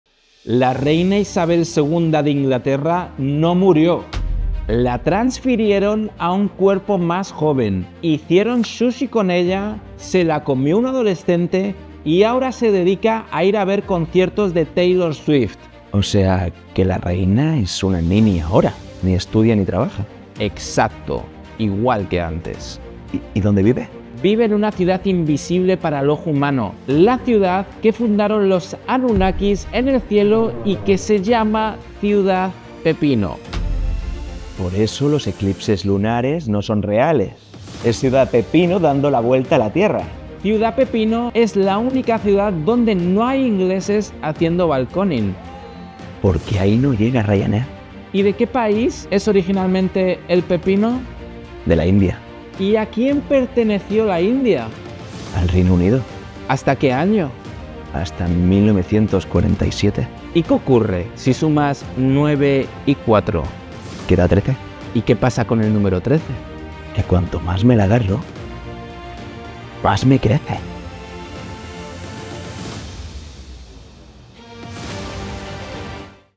La Reina Isabel II de Inglaterra NO MURIÓ ✅
Hicieron sushi con ella y probablemente te la has comido. La reina eres tú.

El sistema te quiere mantener dormido pero poco a poco la verdad sale a la luz.

Con @charesconteresa  🎙️